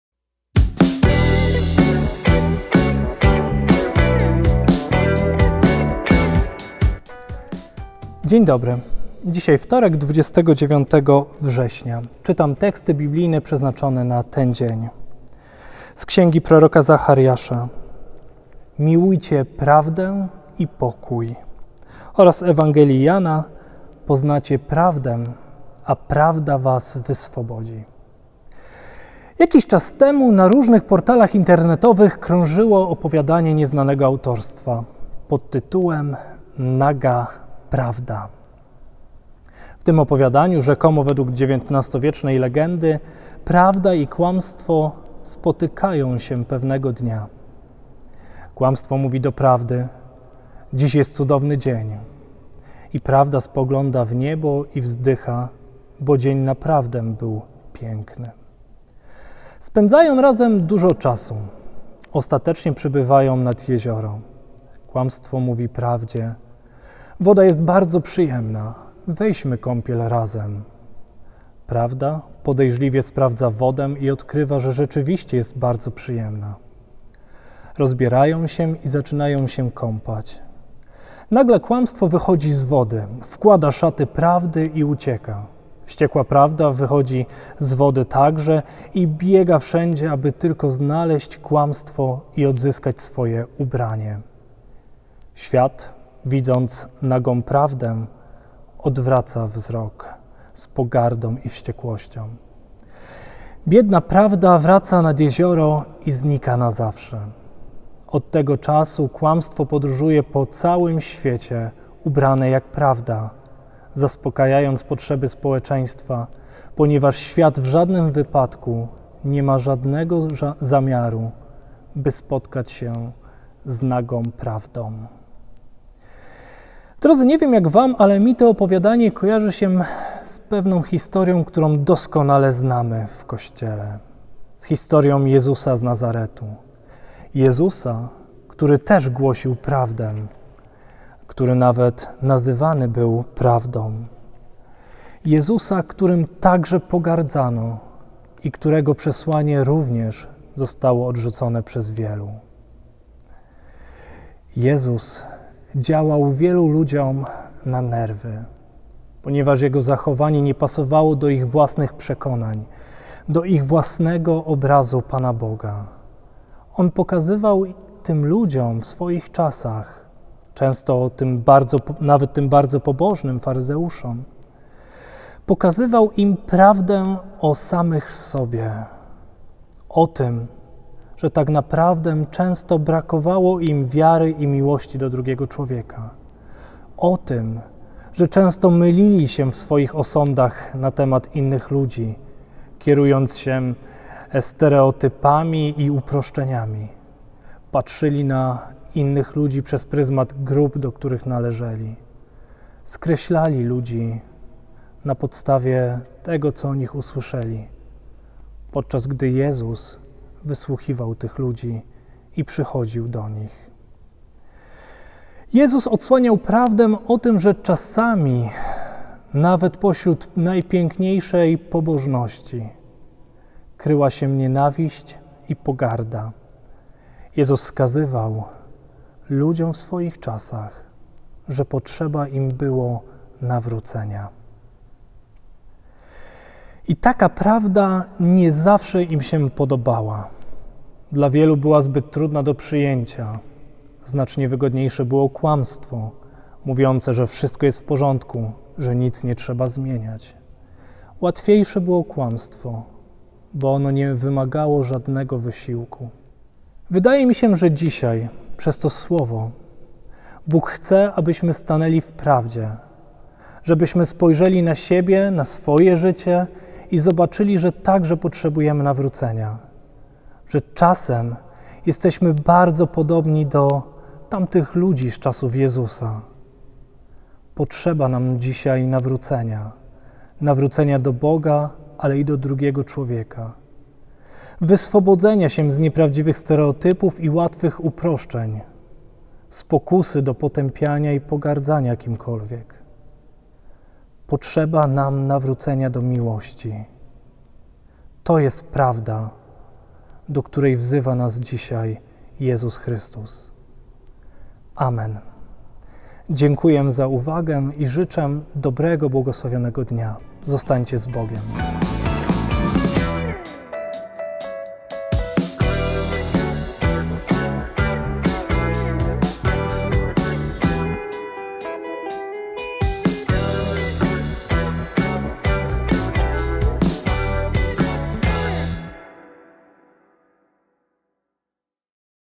Codziennik biblijny, Słowo na dzień 29 września 2020 r.
ks. Arkadiusz Raszka, wikariusz parafii ewangelicko-augsburskiej w Pabianicach